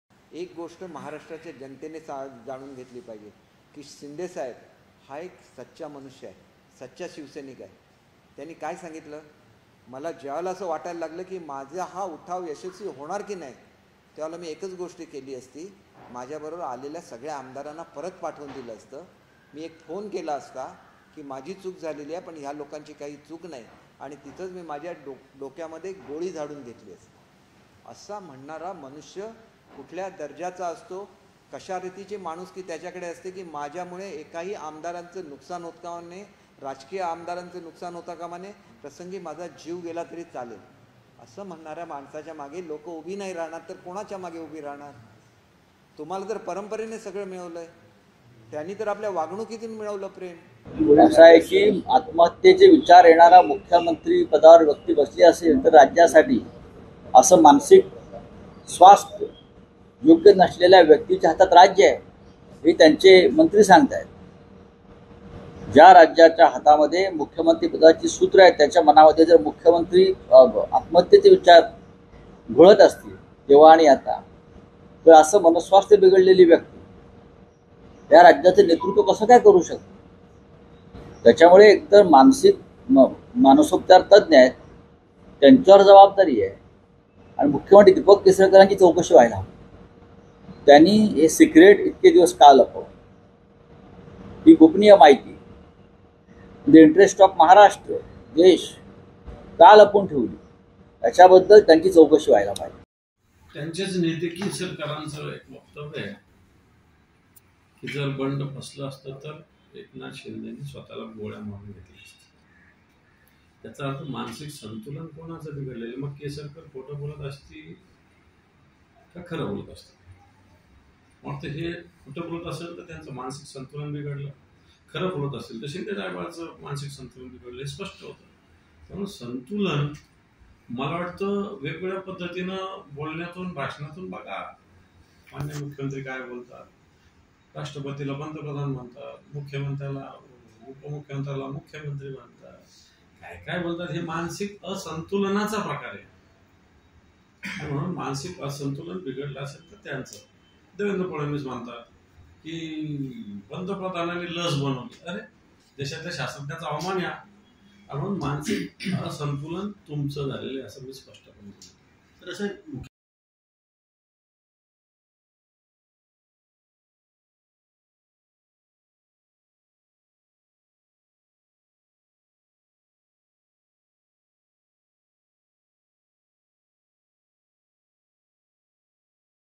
Deepak Kesarkar | केसरकर बोलून गेले, विरोधकांनी चांगलेच घेरले... | LetsUpp Marathi
दीपक केसरकरांच्या विधानावरून विरोधकांनी एकनाथ शिंदेंवर निशाणा साधला.